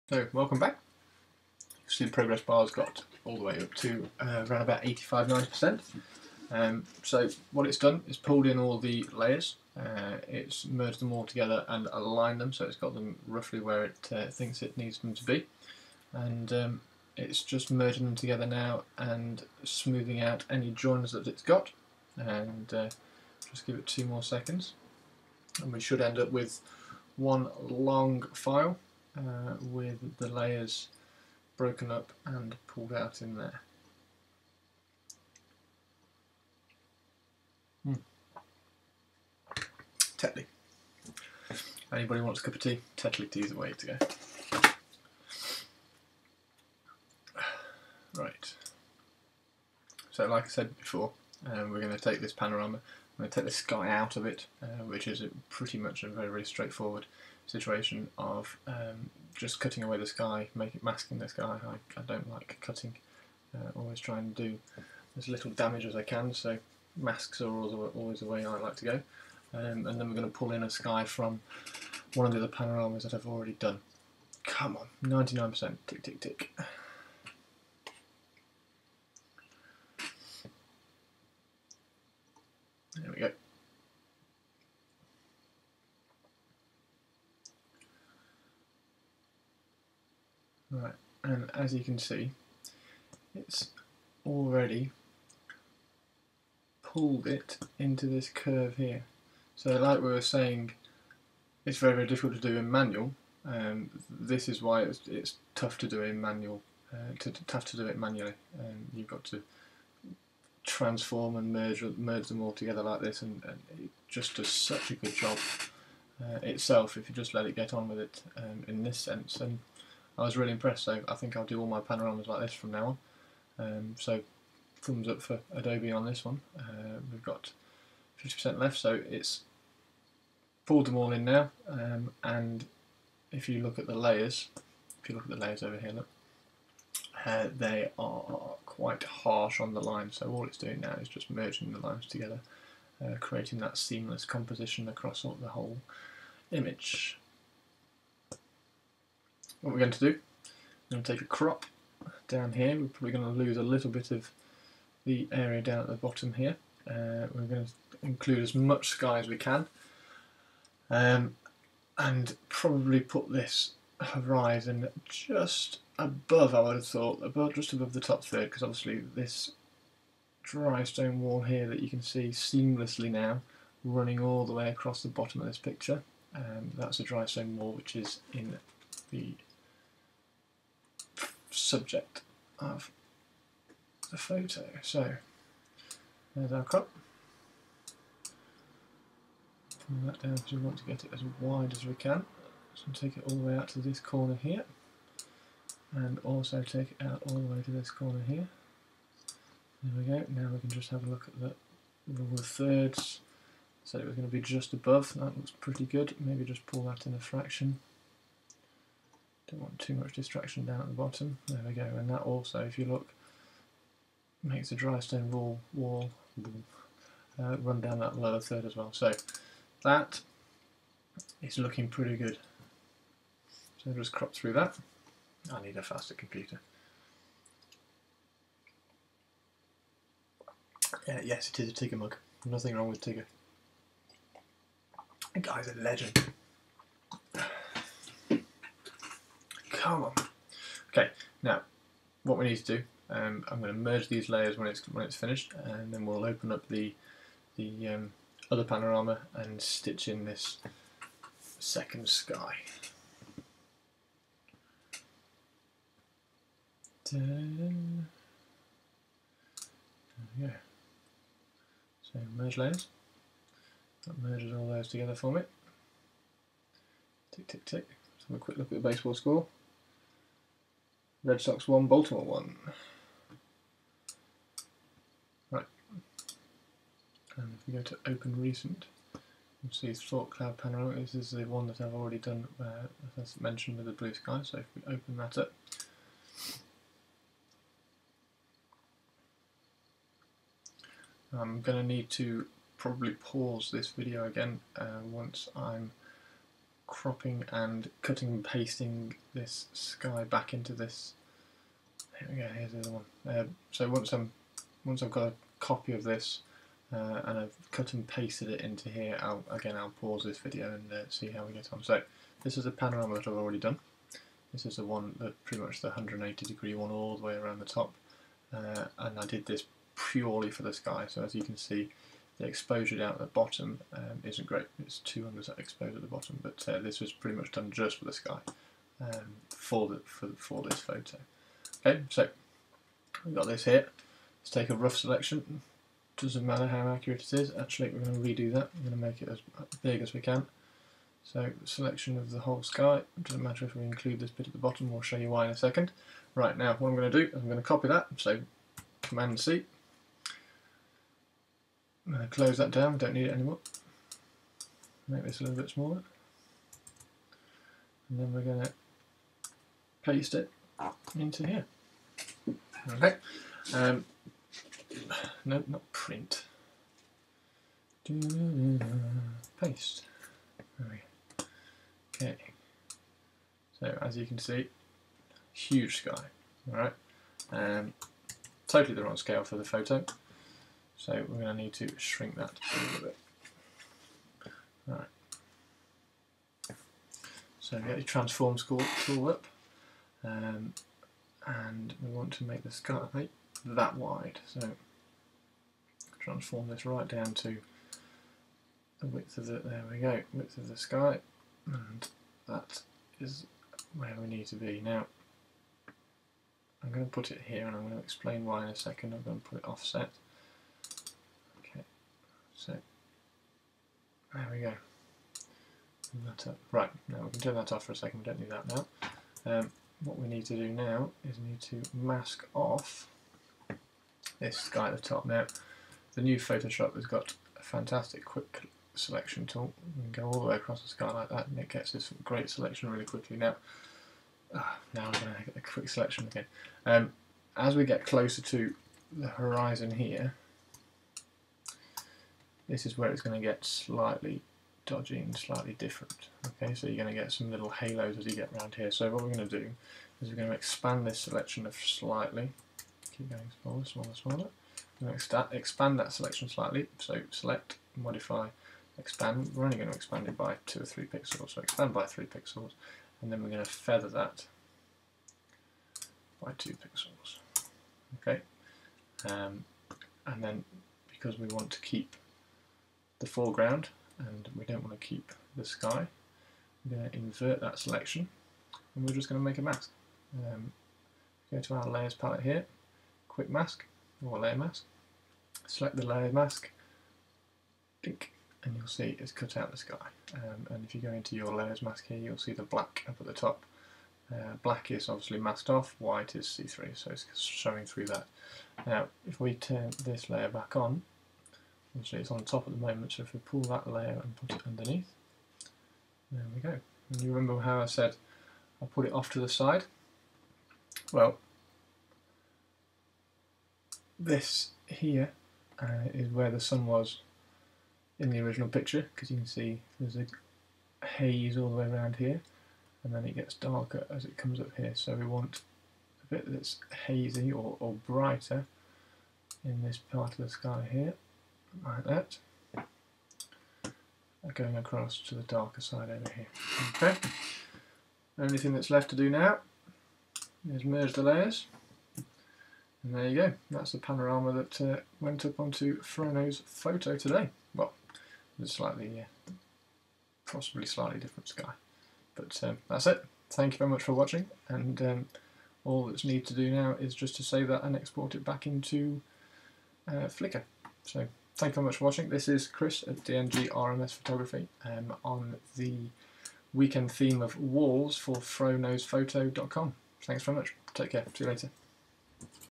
(Pt2) Panorama Tutorial Lightroom to Photoshop
Second part of the video tutorial about how I created the recent panoramic image from the top of Thorpe Cloud in Derbyshire